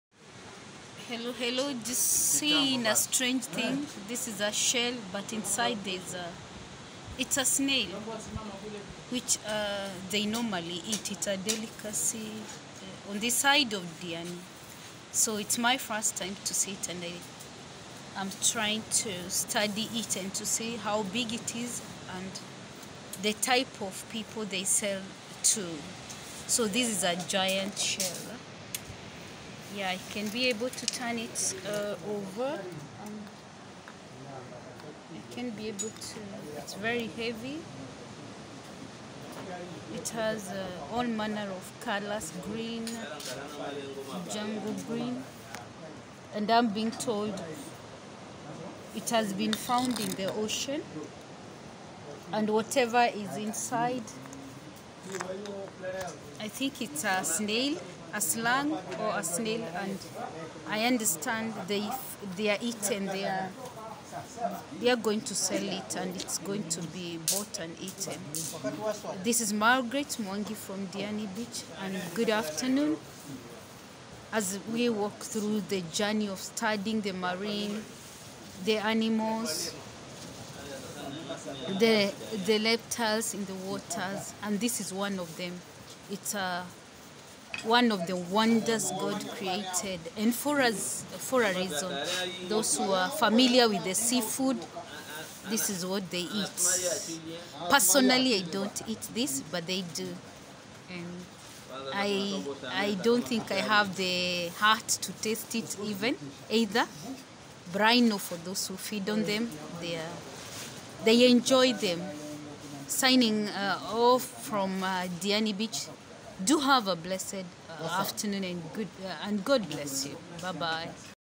Look at what I just found#A giant Cowrie shell with a slug inside#God's wonders in Diani area.
Found a strange shell# God of wonders# Where some sea foods comes from🏖️⛱️. AMAZING WONDERS GLOBAL TRAVEL& DISCOVERY TV CHANNEL.